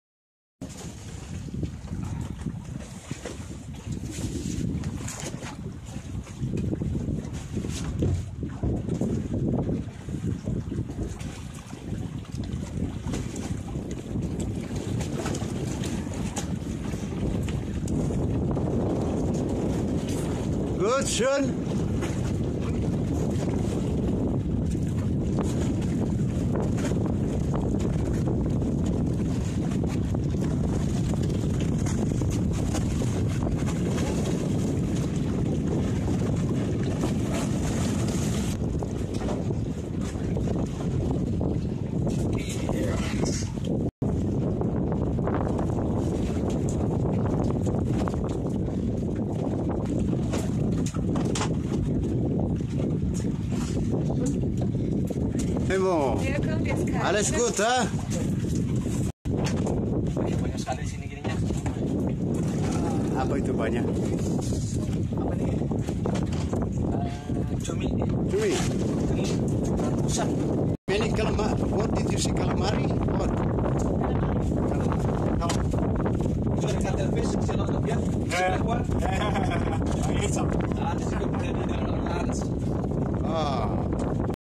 Plongée à l'île de Menjangan avec la famille
L'îleDeMenjangan BaliGuideFrancophone Menjangan SnorkelingBali ParcNationalBaliBarat Bienvenue sur notre chaîne! Merci d'avance ! Cette vidéo vous donne idée de visiter l'île Menjangan avec la famille.